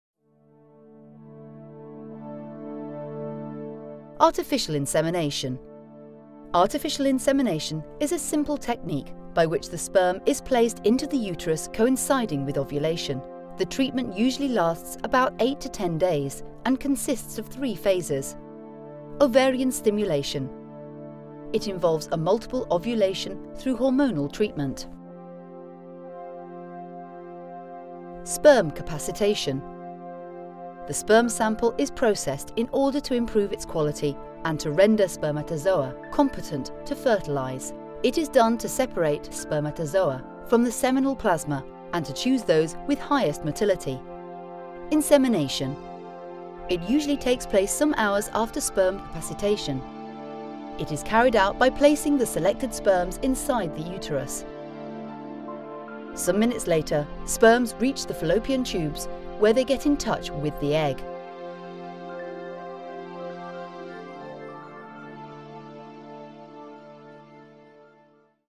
What is artificial insemination?
Ginefiv tells you about the three phases of an artificial insemination: ovarian stimulation, sperm capacitation and insemination. It’s one of the most widely practiced assisted reproduction techniques and it usually takes between 8 and 10 days.

Artificial insemination is an assisted reproduction technique recommended for young couples with fertility problems, among other cases. Make a consultation with Ginefiv and solve all your doubts and questions about our reproduction techniques.